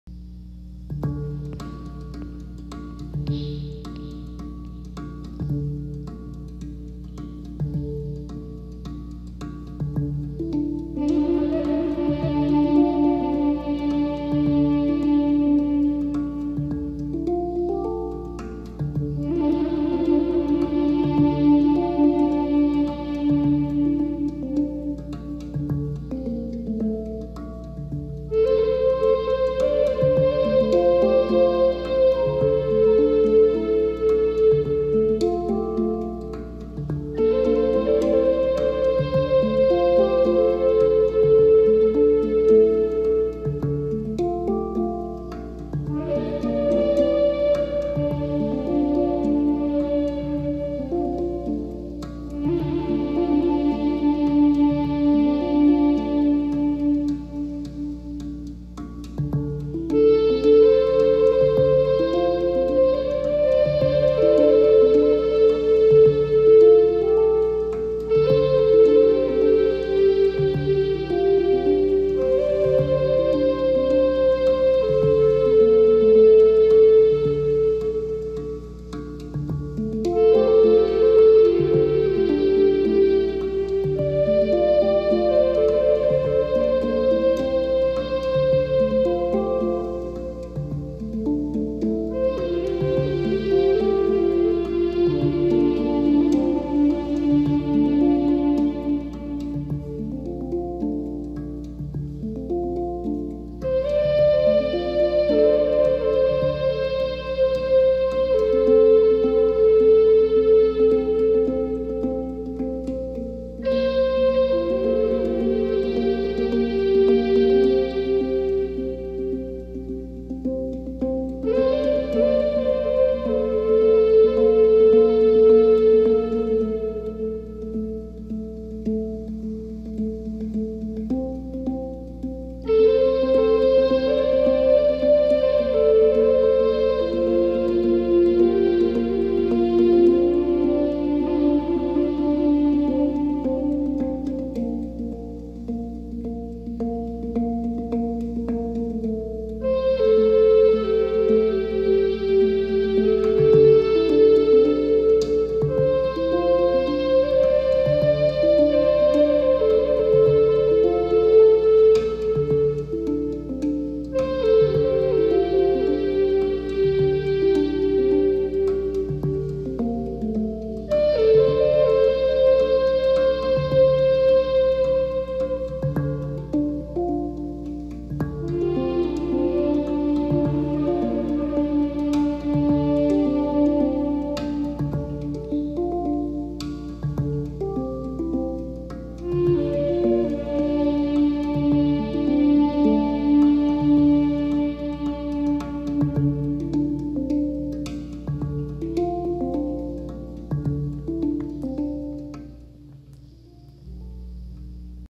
Native American Style Flute Improvisation to Handpan Percussion Backing Track Low D minor
SingingTree Flutes craftsmanship!